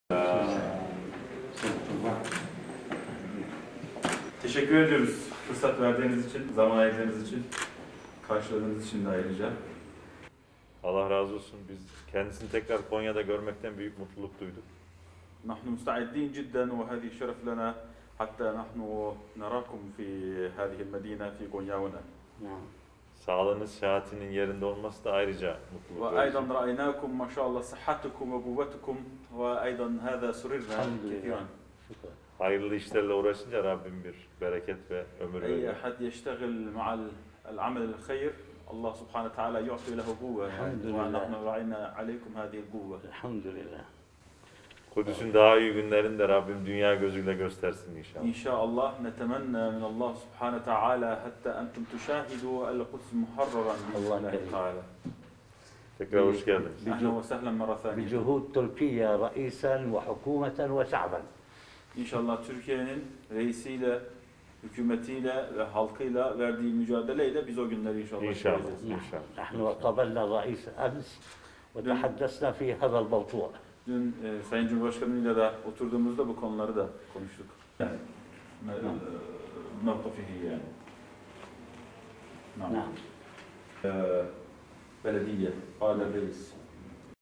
Mescid-i Aksa İmam Hatibi Başkan Altay’ı Ziyaret Etti
Mescid-i Aksa İmam Hatibi ve Kudüs Yüksek İslam Heyeti Başkanı Şeyh İkrime Sabri, Konya Büyükşehir Belediye Başkanı Uğur İbrahim Altay’ı ziyaret etti.
Cumhurbaşkanı Recep Tayyip Erdoğan’ın davetlisi olarak Türkiye’ye gelen Mescid-i Aksa İmam Hatibi ve Kudüs Yüksek İslam Heyeti Başkanı Şeyh İkrime Sabri, Konya Büyükşehir Belediye Başkanı Uğur İbrahim Altay’ı ziyaret etti.
İmam Şeyh İkrime Sabri, Cumhurbaşkanı Erdoğan ile görüşmelerinde Filistin’i ve Kudüs’ün durumunu istişare ettiklerini belirterek, “Türkiye hükümetinin ve halkının verdiği mücadele ile o güzel günleri de göreceğimize inanıyorum.” diye konuştu.
Ziyaretten dolayı büyük memnuniyet duyduğunu dile getiren Başkan Altay ise, “Kudüs'ün daha iyi günlerini de Rabbim bizlere göstersin inşallah.” dedi.